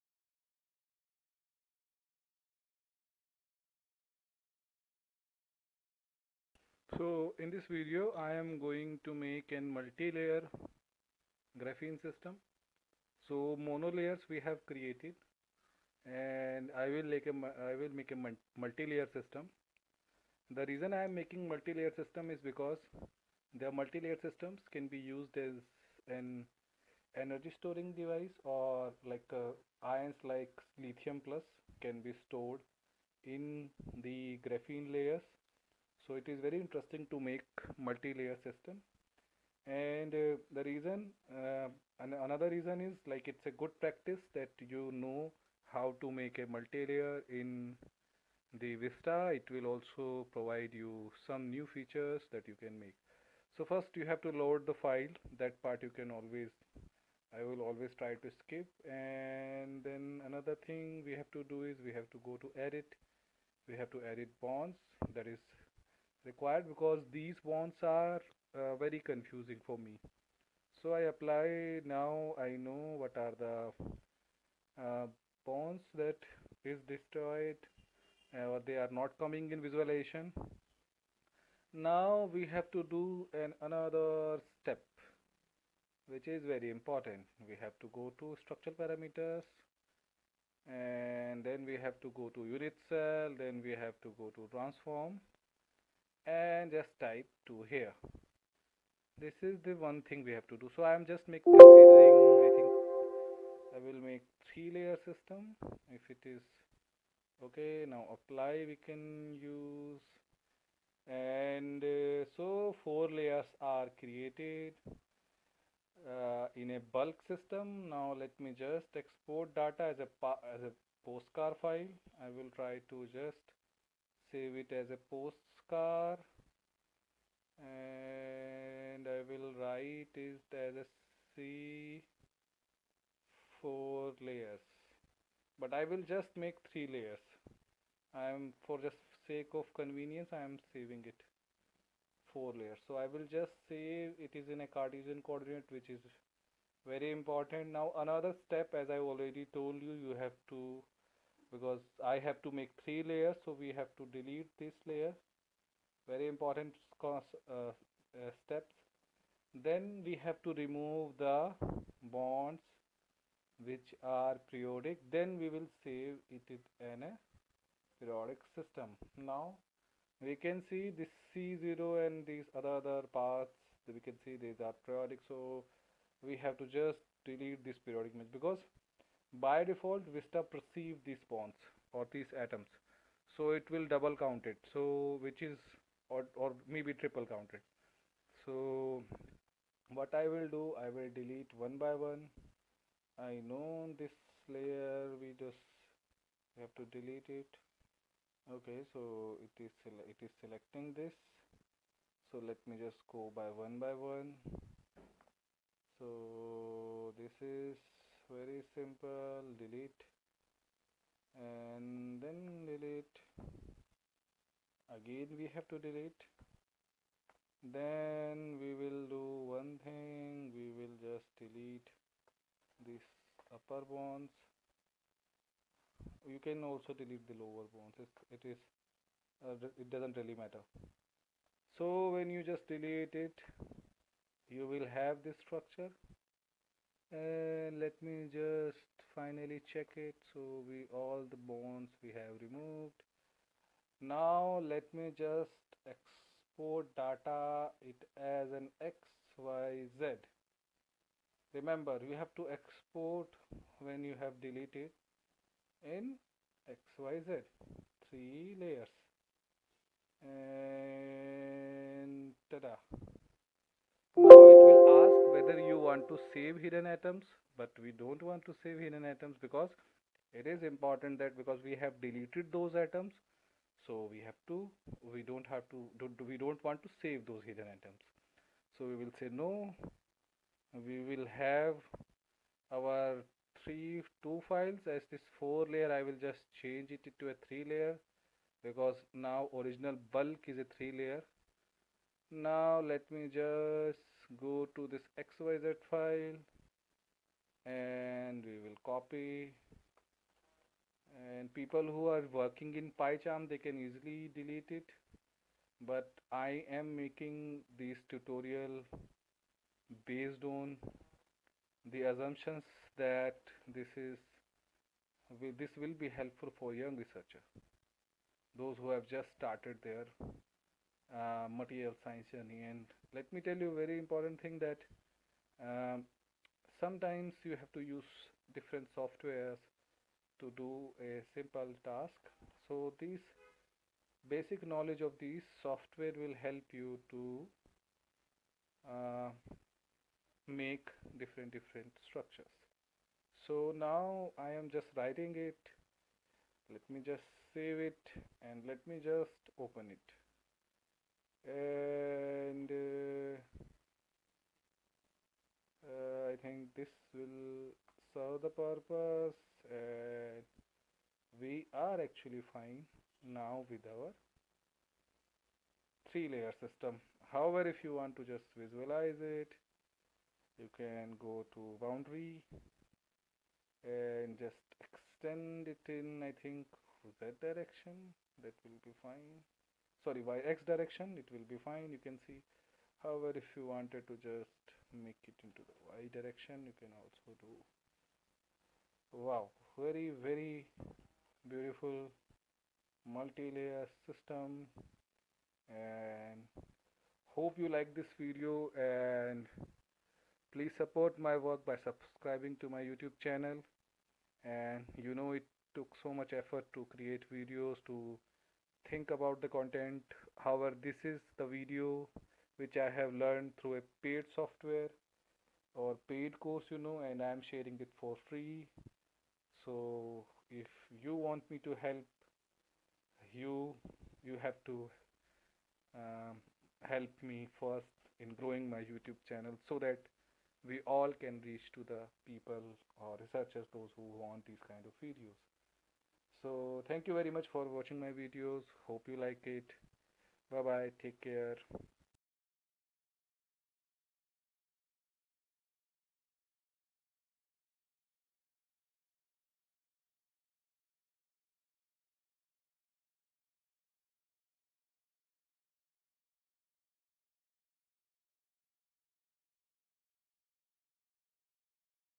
Graphene multilayer creation using VESTA
In this video am creating graphene 3 layers system using VESTA